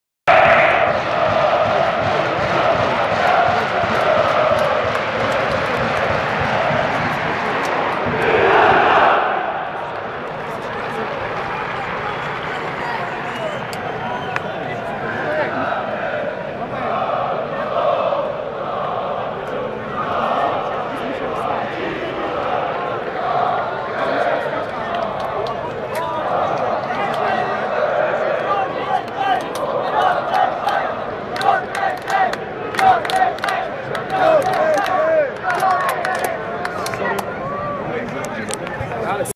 Lech Poznań UEFA Cup
Piotr Reiss warm up before Grasshopers Zurich